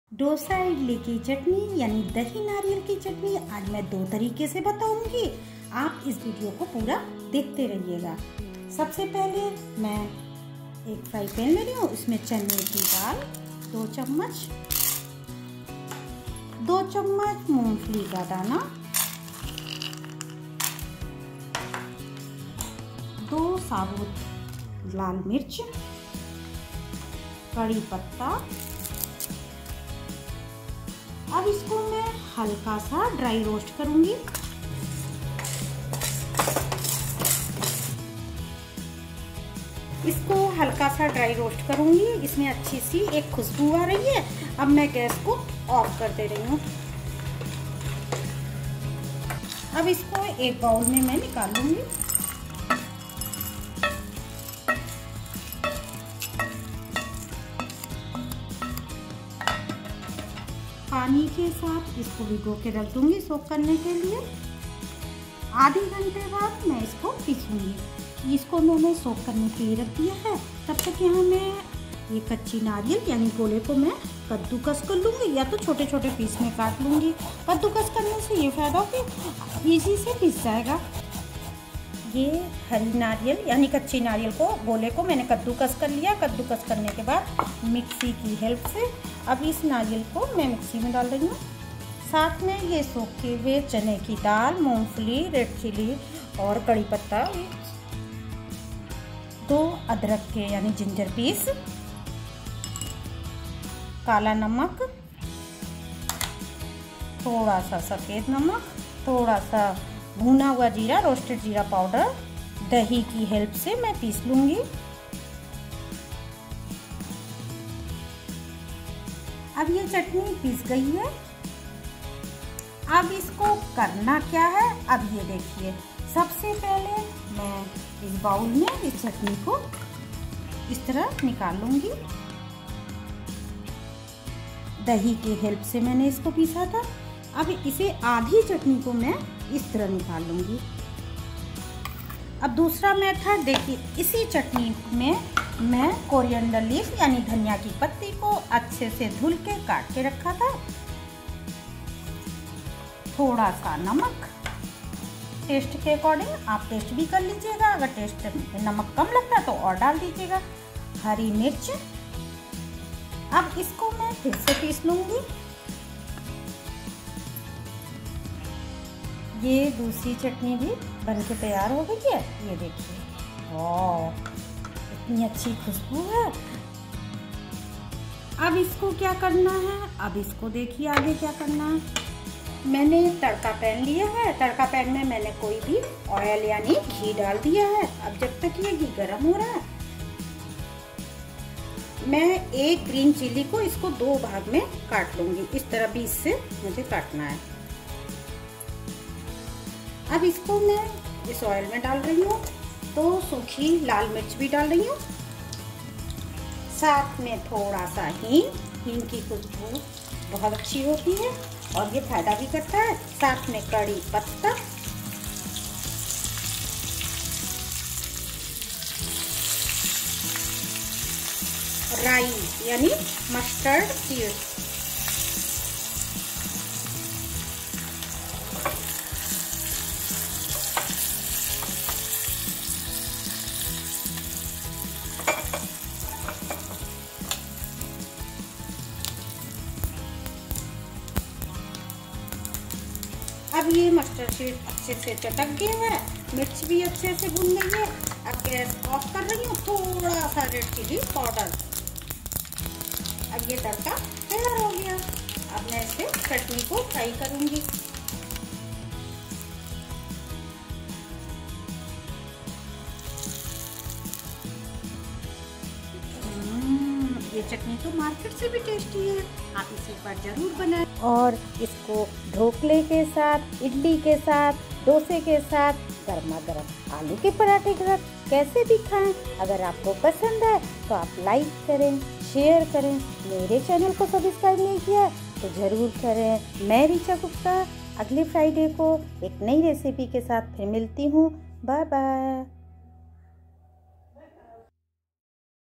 इडली डोसा वाली नारियल की चटनी 2 तरीके से बनाएं | Homemade Coconut Nariyal Chutney Recipe
Welcome to @GharkaMagic !!!

Richa Gupta prepares nariyal ki chutney in two different special ways.
This coconut chutney is one of the healthy recipe which is best served with dosa, idli, paratha and many more food stuffs.
This chutney can be prepare at home very easily and is much more tasty and hygienic than market made chutney.

Watch the video till end and learn how to prepare idli dosa wali best chutney at home quickly and very easily.